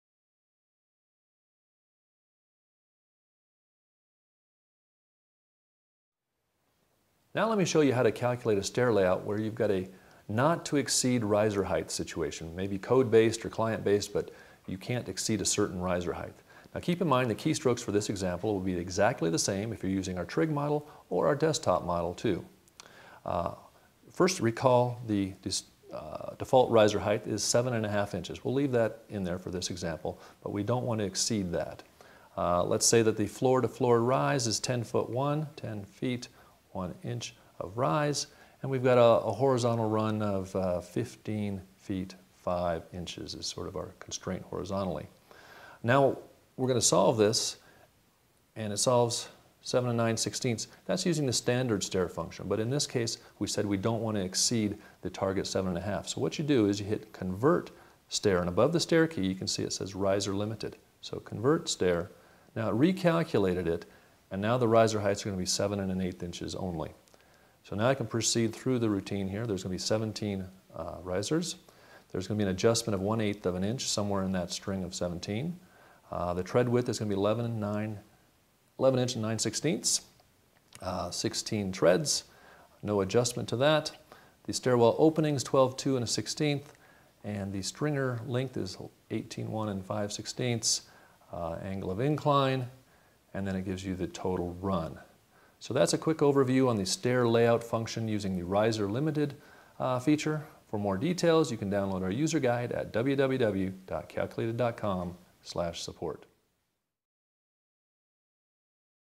How to Calculate Stair Layout with Fixed Riser Height | Construction Master Pro
See how the Construction Master Pro contractor calculator's built-in stair layout key can give you all the answers you need to build your next stairs with a not-to-exceed riser height; including number of risers, riser height, number of treads, tread widths, stringer length, angle of incline and total run of the stairs. This quick demo briefly shows how to use this construction math power tool to do all your design, estimating and jobsite calculations far more quickly, simply and accurately to improve your productivity and profitability.

Construction Master® and Calculated Industries® are registered trademarks of Calculated Industries, Inc.